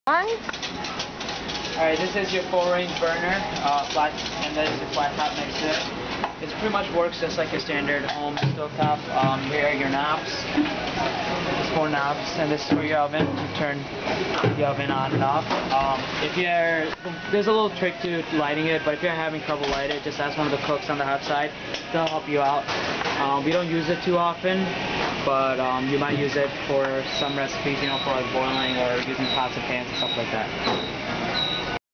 4 Range Burner
4 Range Burner/ Flat top
Notes

-A little tricky to light; have to give a little poof of air to get it to light or use a match.